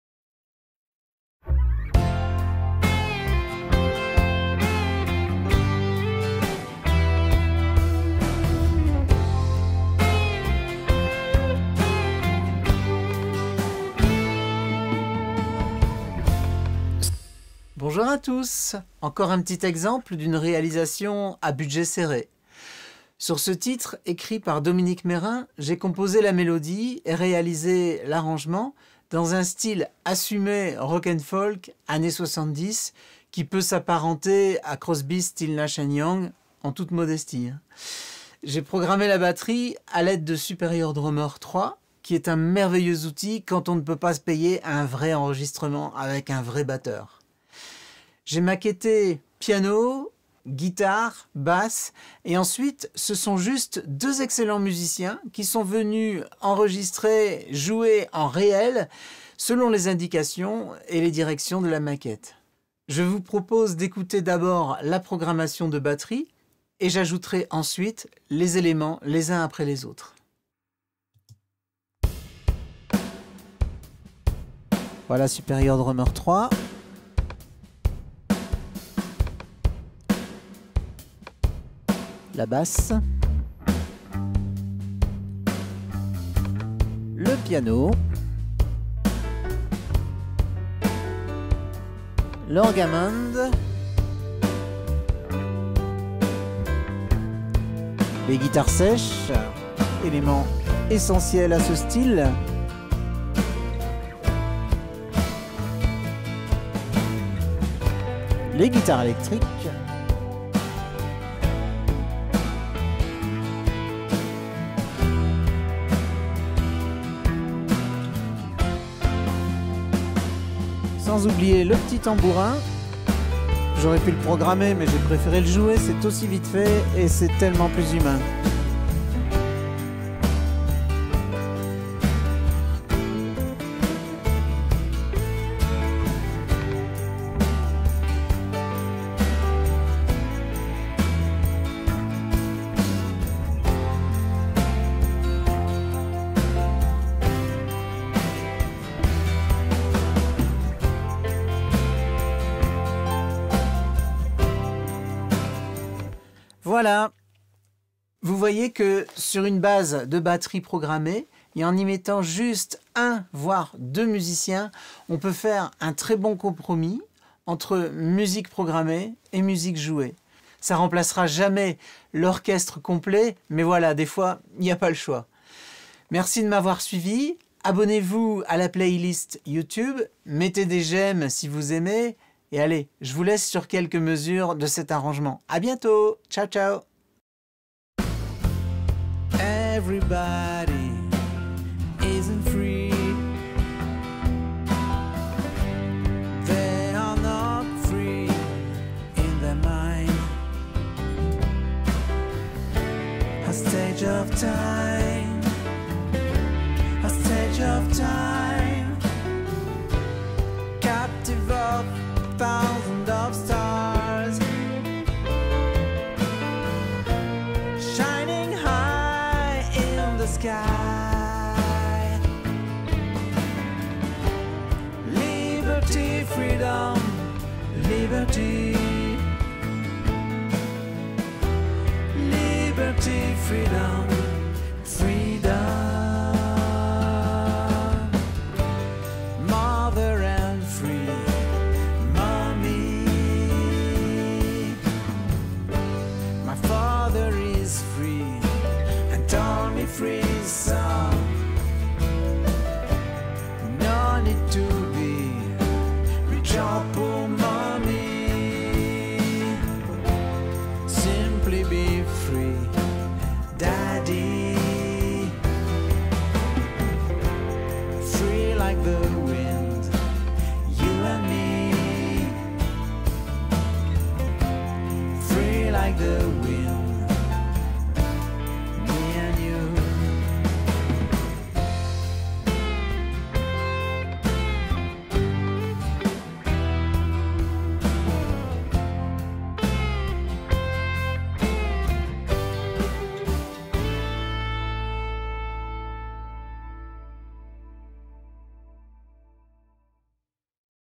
Exemple 2 - Sur un texte de Dominique Mérin
ll arrive que l'on vienne nous trouver avec juste un texte, quelques bribes de mélodie et… un petit budget.
Sur un texte de Dominique Mérin, voici un exemple de composition et d'arrangements de Christophe Berly.
Sur les programmations de Christophe Berly guitares claviers ont ensuite été joués live au studio avec le guitariste Joël Gombert et le pianiste Christian Mornet.